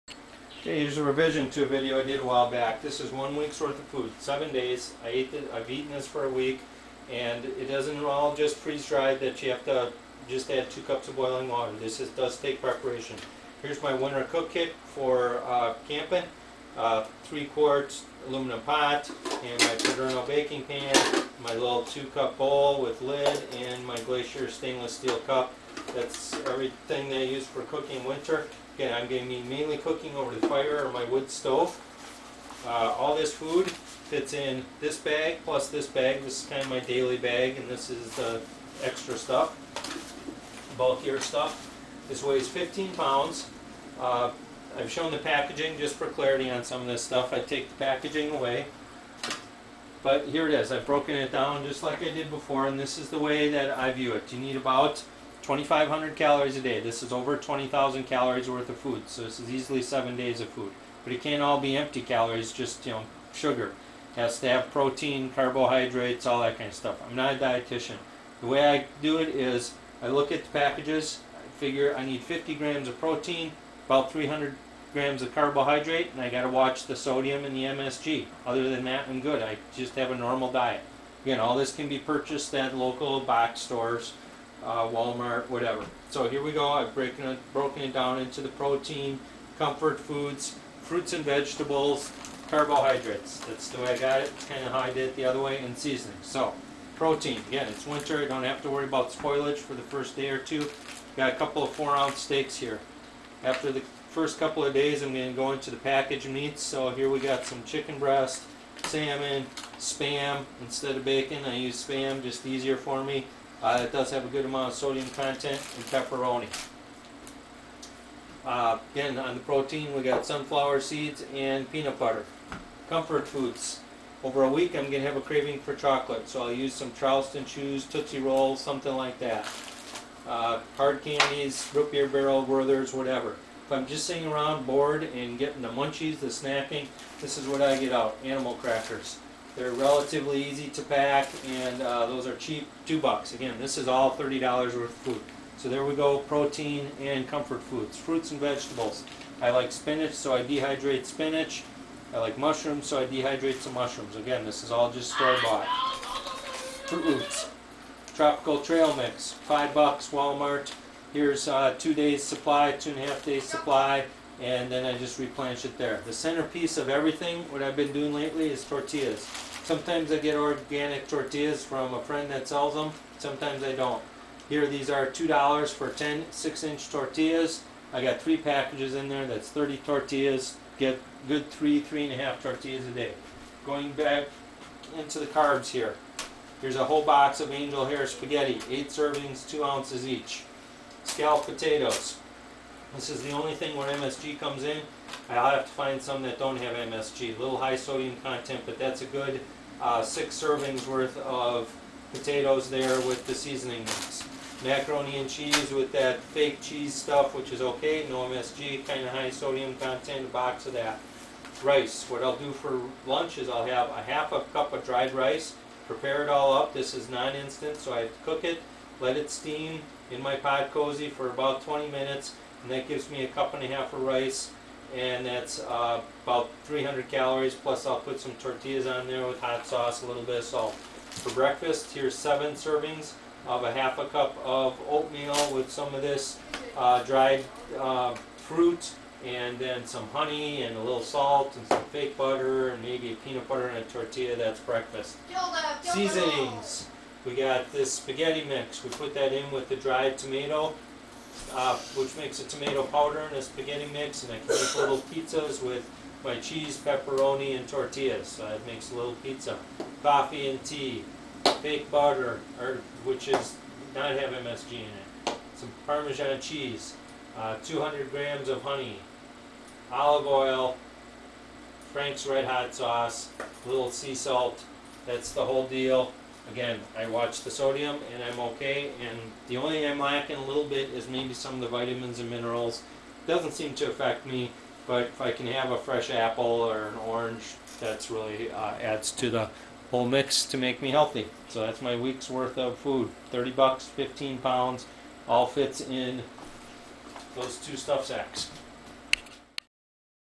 1 week food list - revised
Here is a list of the food that I prepare per a 1 week grocery trip. All of this food can be easily cooked on my woodburning stove.  It weighs 15# cost $30 and is over 20,000 calories.  It all fits into a 15Liter bag + a 3 liter bag. "This is not "freeze dried" boil and add water.  Some of this food requires preparation and cooking